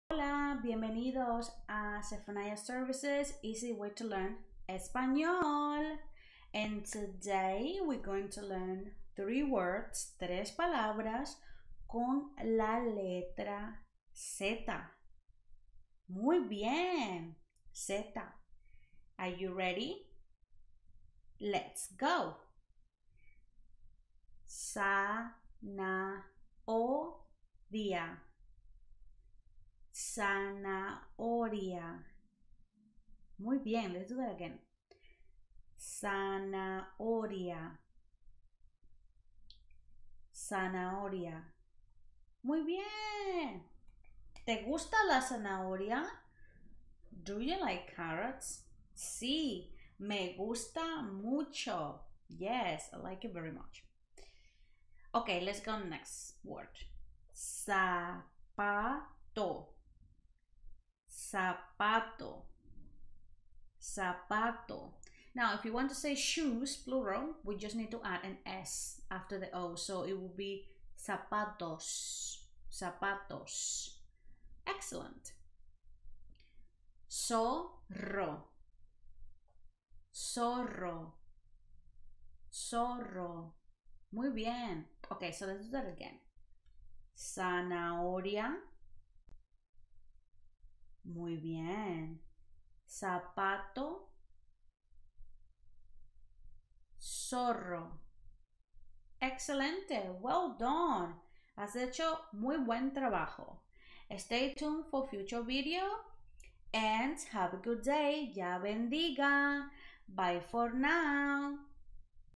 Spanish Alphabet + Vocabulary - Letter Z
Hello and Welcome to ZephaniYah Services, EasyWaytoLearn Spanish🇪🇸
🆕 "Alphabet + Vocabulary lesson series
Where you will learn 3 words per letter.
Today we will go over the Letter Z:
➡️Zanahoria.
➡️Zapato.
➡️Zorro.